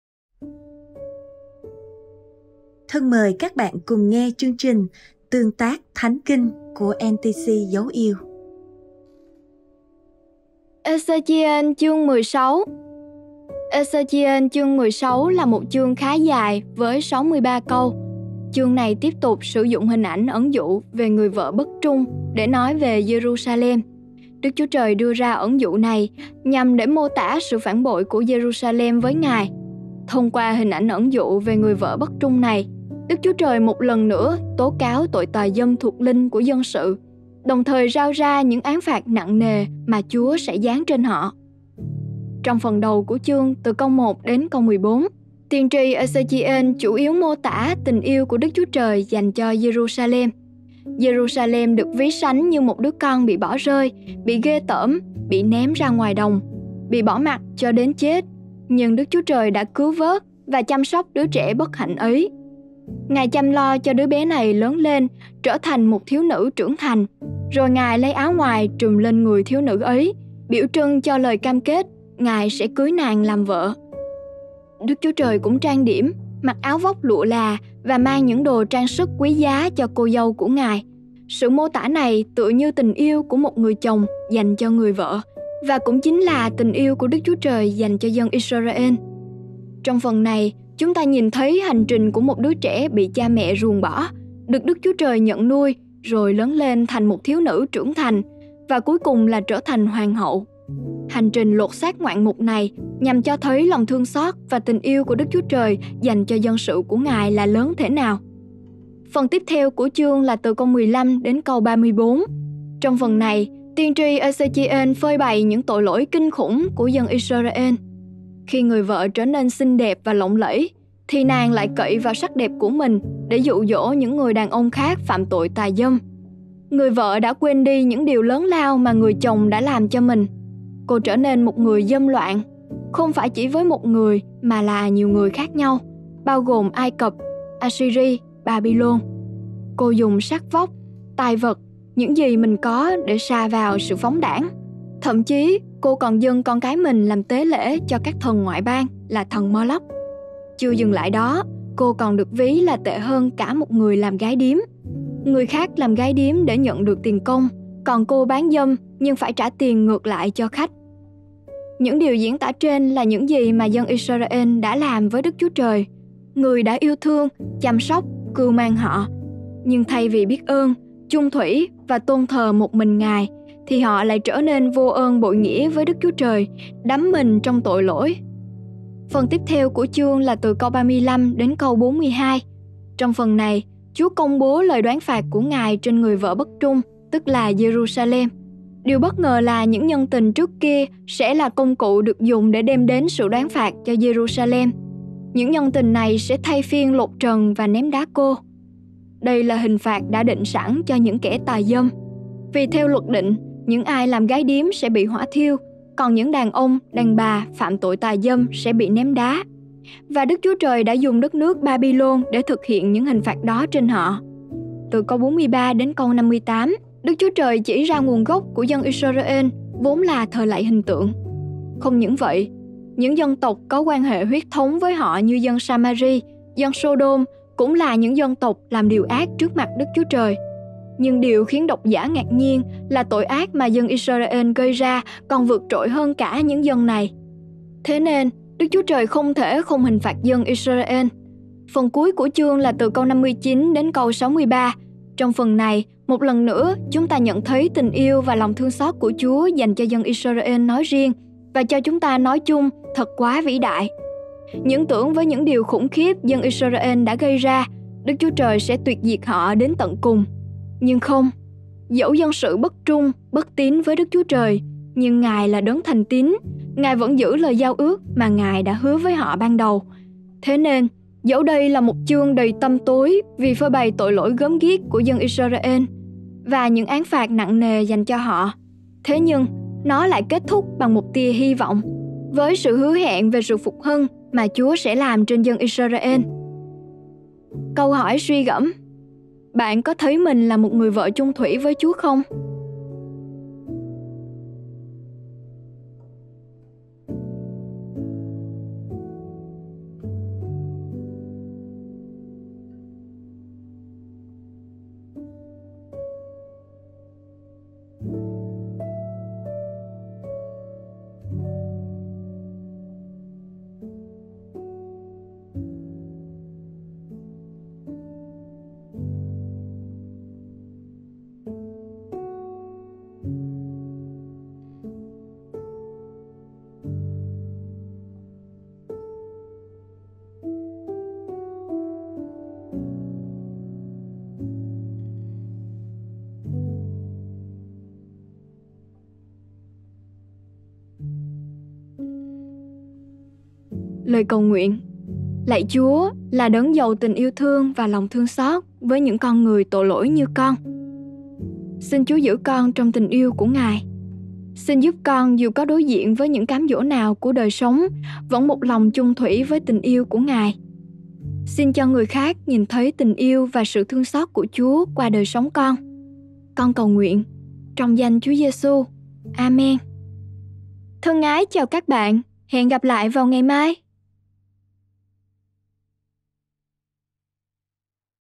[TƯƠNG TÁC THÁNH KINH] 💦 Ê-XÊ-CHI-ÊN 16 🛐
Chương trình TƯƠNG TÁC THÁNH KINH
Kinh thánh: Ê-XÊ-CHI-ÊN 16
🌿 Bài viết: Nhóm Thông Công
🌿 Giọng đọc: Bích Phượng

🌱 Đọc thêm 1 bản dịch khác.
🎧 Lắng nghe bài phản hồi

💦 Ê-xê-chi-ên chương 16 là một chương khá dài, với 63 câu. Chương này tiếp tục sử dụng hình ảnh ẩn dụ về người vợ bất trung để nói về Giê-ru-sa-lem. Đức Chúa Trời đưa ra ẩn dụ này nhằm để mô tả sự phản bội của Giê-ru-sa-lem với Ngài. Thông qua hình ảnh ẩn dụ về người vợ bất trung này, Đức Chúa Trời một lần nữa tố cáo tội ‘tà dâm thuộc linh’ của dân sự, đồng thời rao ra những án phạt nặng nề mà Chúa sẽ giáng trên họ.

💦 Trong phần đầu của chương từ câu 1 đến câu 14, tiên tri Ê-xê-chi-ên chủ yếu mô tả tình yêu của Đức Chúa Trời dành cho Giê-ru-sa-lem. Giê-ru-sa-lem được ví sánh như một đứa con bị bỏ rơi, bị ghê tởm, bị ném ra ngoài đồng, bị bỏ mặc cho đến chết. Nhưng Đức Chúa Trời đã cứu vớt và chăm sóc đứa trẻ bất hạnh ấy. Ngài chăm lo cho đứa bé này lớn lên, trở thành một thiếu nữ trưởng thành. Rồi Ngài lấy áo ngoài trùm lên người thiếu nữ ấy, biểu trưng cho lời cam kết Ngài sẽ cưới nàng làm vợ. Đức Chúa Trời cũng trang điểm, mặc áo vóc lụa là, và mang những đồ trang sức quý giá cho cô dâu của Ngài. Sự mô tả này tựa như tình yêu của một người chồng dành cho người vợ, và cũng chính là tình yêu của Đức Chúa Trời dành cho dân Y-sơ-ra-ên. Trong phần này chúng ta nhìn thấy hành trình của một đứa trẻ bị cha mẹ ruồng bỏ, được Đức Chúa Trời nhận nuôi, rồi lớn lên thành một thiếu nữ trưởng thành, và cuối cùng là trở thành hoàng hậu. Hành trình lột xác ngoạn mục này nhằm cho thấy lòng thương xót và tình yêu của Đức Chúa Trời dành cho dân sự của Ngài là lớn thể nào!

💦 Phần tiếp theo của chương là từ câu 15 đến câu 34. Trong phần này, tiên tri Ê-xê-chi-ên phơi bày những tội lỗi kinh khủng của dân Y-sơ-ra-ên. Khi người vợ trở nên xinh đẹp và lộng lẫy, thì nàng lại cậy vào sắc đẹp của mình để dụ dỗ những người đàn ông khác phạm tội tà dâm. Người vợ đã quên đi những điều lớn lao mà người chồng đã làm cho mình. Cô trở nên một người dâm loạn, không phải chỉ với một người mà là nhiều người khác nhau, bao gồm: Ai-cập, A-si-ri, Ba-by-lôn. Cô dùng sắc vóc, tài vật những gì mình có để sa vào sự phóng đãng. Thậm chí cô còn dâng con cái mình làm tế lễ cho các thần ngoại bang là thần Mo-lóc.

💦 Chưa dừng lại đó, cô còn được ví là tệ hơn cả một người làm gái điếm. Người khác làm gái điếm để nhận được tiền công, còn cô bán dâm nhưng phải trả tiền ngược lại cho khách. Những điều diễn tả trên là những gì mà dân Y-sơ-ra-ên đã làm với Đức Chúa Trời. Người đã yêu thương, chăm sóc, cưu mang họ, nhưng thay vì biết ơn, chung thủy và tôn thờ một mình Ngài, thì họ lại trở nên vô ơn bội nghĩa với Đức Chúa Trời, đắm mình trong tội lỗi.

💦 Phần tiếp theo của chương là từ câu 35 đến câu 42. Trong phần này, Chúa công bố lời đoán phạt của Ngài trên người vợ bất trung, tức là Giê-ru-sa-lem. Điều bất ngờ là những ‘nhân tình’ trước kia sẽ là công cụ được dùng để đem đến sự đoán phạt cho Giê-ru-sa-lem. Những ‘nhân tình’ này sẽ thay phiên lột trần và ném đá cô. Đây là hình phạt đã đã định sẵn cho những kẻ tà dâm. Vì theo luật định những ai làm gái điếm sẽ bị hỏa thiêu, còn những đàn ông đàn bà phạm tội tà dâm sẽ bị ném đá. Và Đức Chúa Trời đã dùng đất nước Ba-by-lôn để thực hiện những hình phạt đó trên họ.
Phần tiếp theo của chương là từ câu 43 đến câu 58. Trong phần này, Đức Chúa Trời chỉ ra nguồn gốc của dân Y-sơ-ra-ên vốn là thờ lạy hình tượng. Không những vậy, những dân tộc có quan hệ huyết thống với họ như dân Sa-ma-ri, dân Sô-đôm cũng là những dân tộc làm điều ác trước mặt Đức Chúa Trời. Nhưng điều khiến độc giả ngạc nhiên là tội ác mà dân Y-sơ-ra-ên gây ra còn còn vượt trội hơn cả những dân này. Thế nên, Đức Chúa Trời không thể không hình phạt dân Y-sơ-ra-ên.

💦 Phần cuối của chương là từ câu 59 đến câu 63. Trong phần này, một lần nữa chúng ta nhận thấy tình yêu và lòng thương xót của Chúa dành cho dân Y-sơ-ra-ên nói riêng và cho chúng ta nói chung thật quá vĩ đại. Những tưởng với những điều khủng khiếp dân Y-sơ-ra-ên đã gây ra, Đức Chúa Trời sẽ tuyệt diệt họ đến tận cùng. Nhưng không! Dẫu dân sự bất trung bất tín với Đức Chúa Trời, nhưng Ngài là Đấng thành tín, Ngài vẫn giữ lời giao ước mà Ngài đã hứa với họ ban đầu. Thế nên, dẫu đây là một chương đầy tăm tối vì phơi bày tội lỗi gớm ghiếc của dân Y-sơ-ra-ên, và những bán án phạt nặng nề dành cho họ. Thế nhưng nó lại kết thúc bằng một tia hy vọng, với sự hứa hẹn về sự phục hưng mà Chúa sẽ làm trên dân Y-sơ-ra-ên.

⏳⏳⏳CÂU HỎI SUY NGẪM:
Bạn có thấy mình là một ‘người vợ’ chung thủy với Chúa không❓